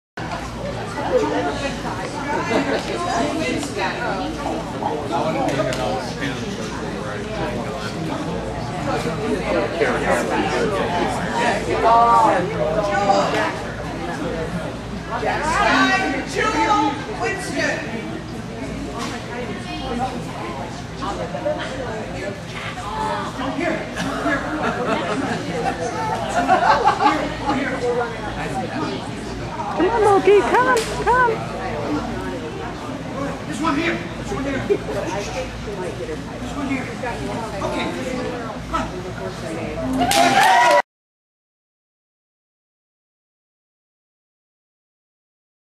Loki's First Trial
First trial at Queen City Dog Training Club on 6/17/07.